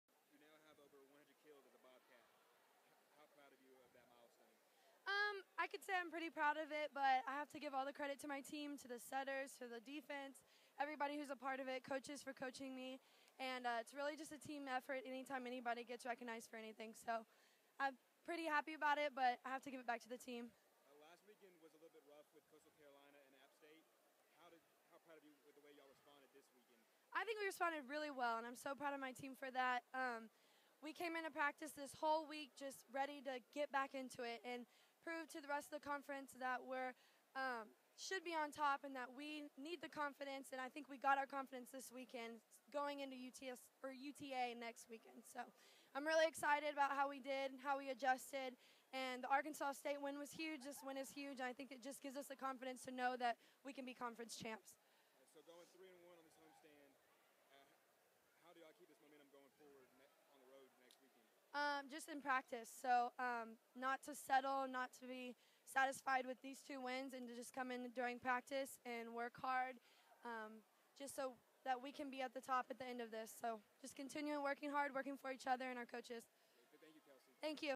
Volleyball SR Kelsey Weynand-Postgame Little Rock
Senior Kelsey Weynand provides her thoughts on Texas State winning two matches this weekend, including tonight's 3-0 victory Little Rock.